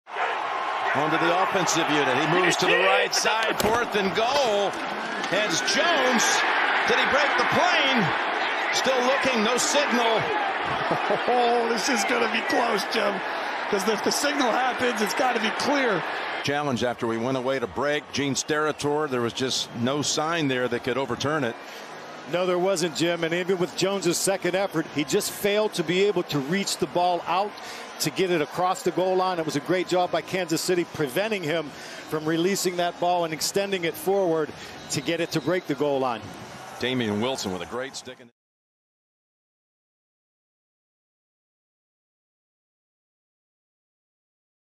Chiefs Stop Bucs on 4th & Goal
The Chiefs defensive line manages to hold Ronald Jones and the Bucs out of the end zone on 4th down. The Kansas City Chiefs take on the Tampa Bay Buccaneers during Super Bowl LV in Tampa Bay.